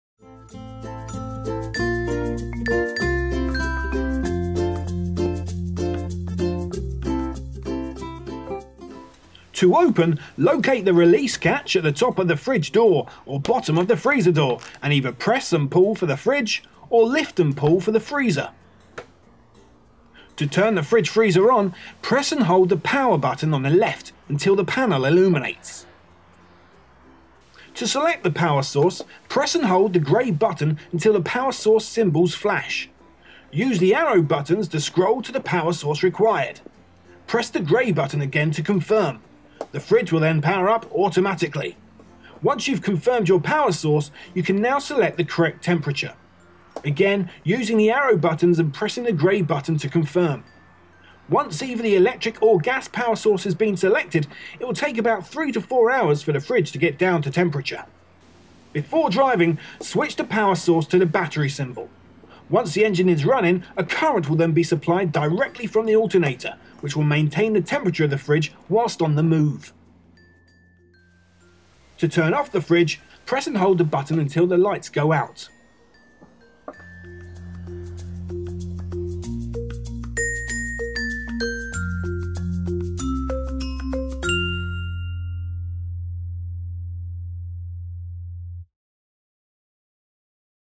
Fridge/Freezer
How to use the fridge/freezer in one of our Chausson motorhomes